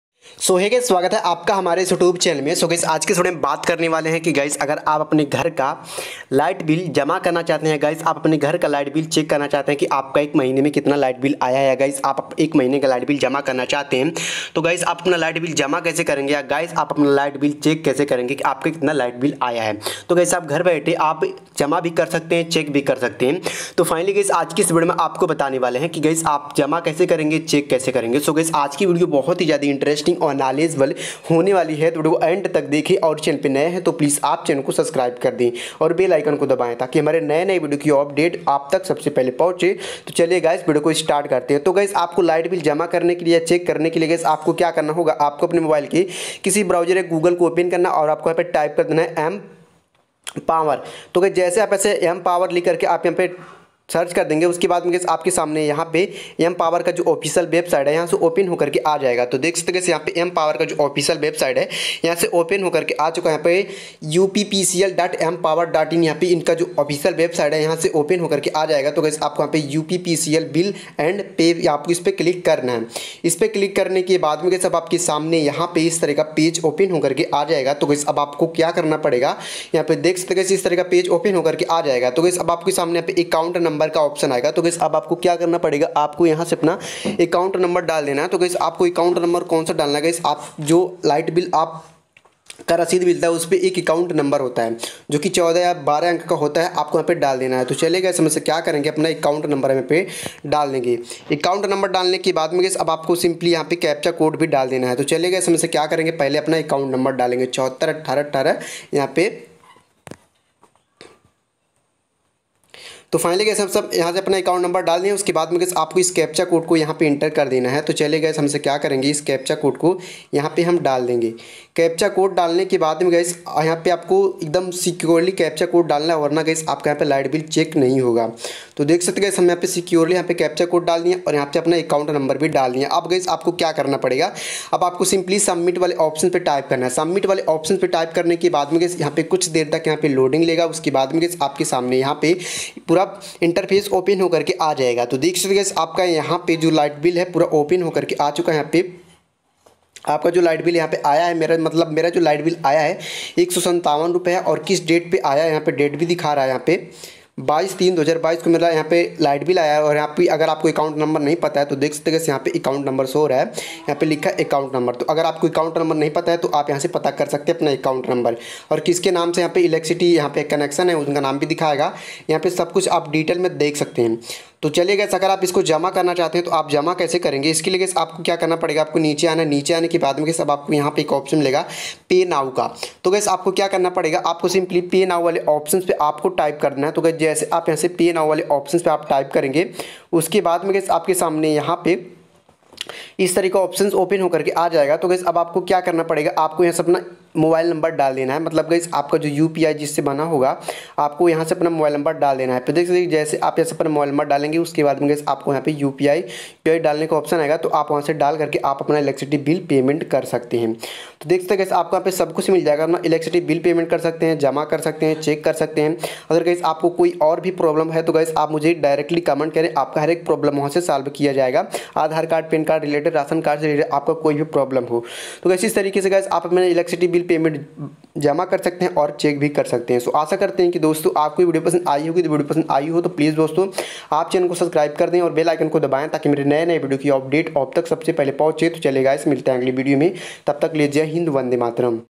बिजली बिल कैसे चेक करें 2022 l Bijli Bill kaise Pay kaise kare l how to pay Bijli Bill in hindi
Hey Guys Aaj ke patthe s video me ki aap Bijli Bill kaise check kare aor kaise pay kare